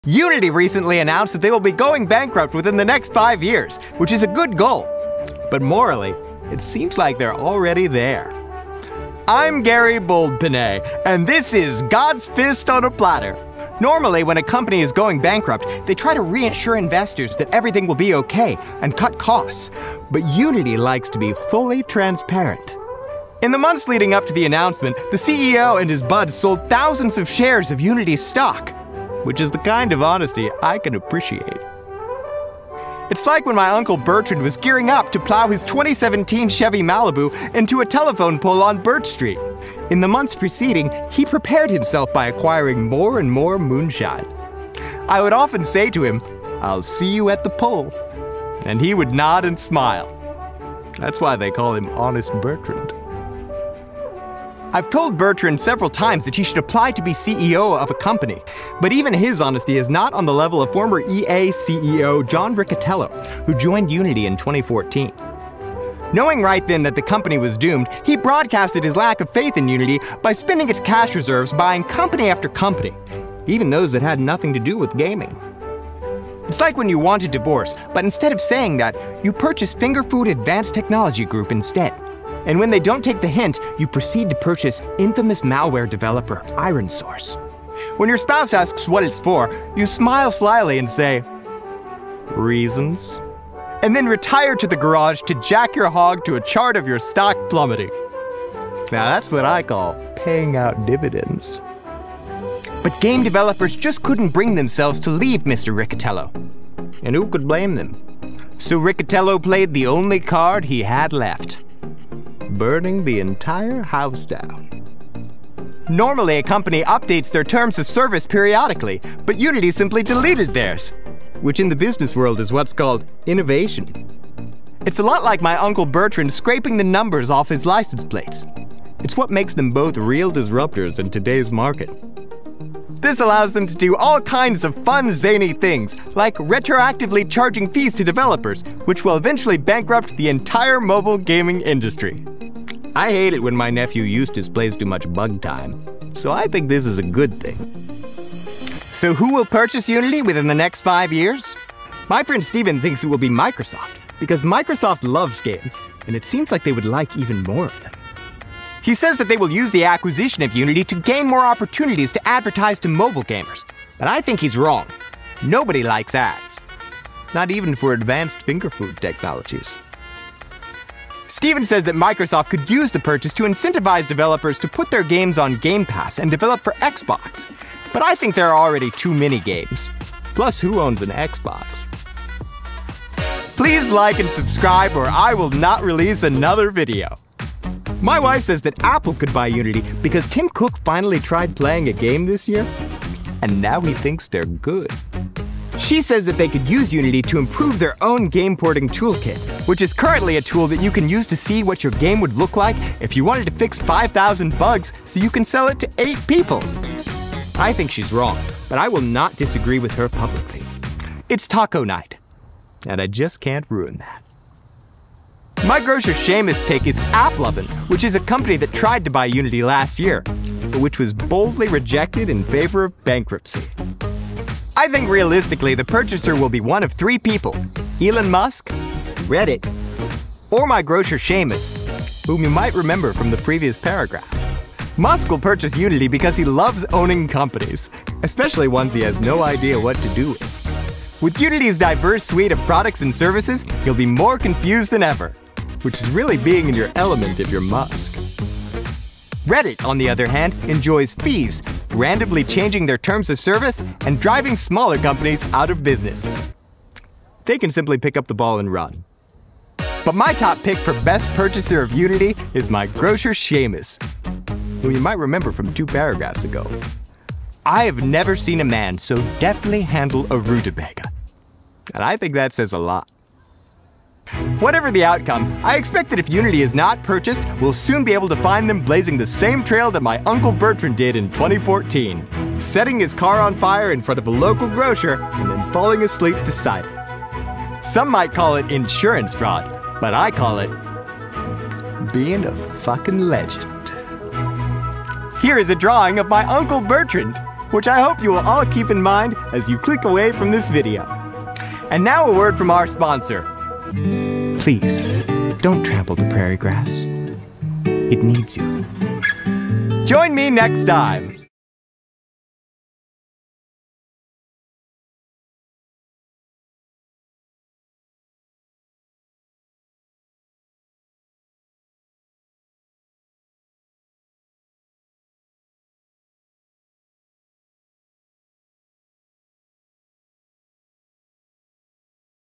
Unity Fee Drama: Who Buys Them When They Go Bankrupt In A Year?
Unity recently announced new runtime fees for game developers. We respond to John Riccitiello's bold announcement of upcoming bankruptcy by speculating on which company will buy them in the near future.

The Garrhy Boldpenis Program - Episode 3: God's Fist on a Platter
Produced by Justin Norman and Elissa Rippey
Camera: Elissa Rippey and Ethan Nelson